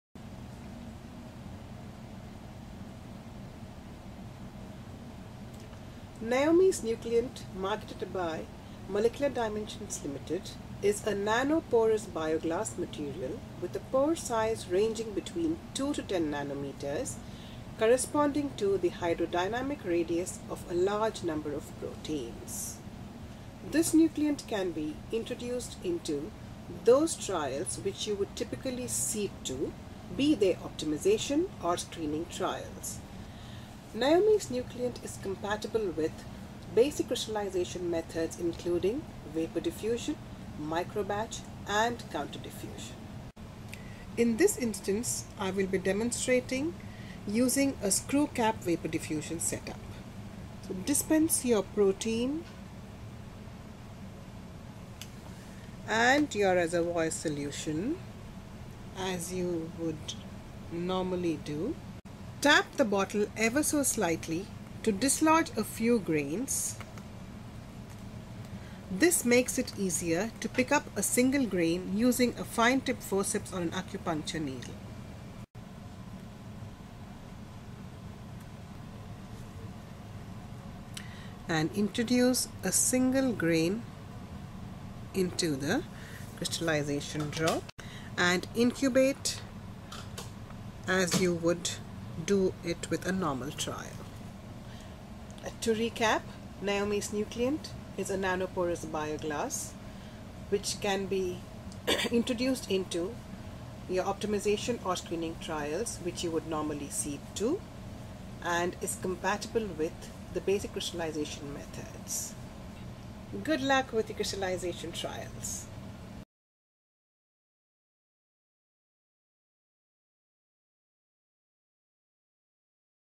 How to use Naomi's Nucleants to enhance your protein crystallization experiments.
Having problems getting larger protein crystals? Still getting precipitate? Just can't get that elusive protein crystal? Then material they use in maxillofacial reconstruction, which provide the perfect nucleation site for your protein crystals to grow. They encourage 'ordered' crystal growth thereby increasing your chances of obtaining better diffracting protein crystals.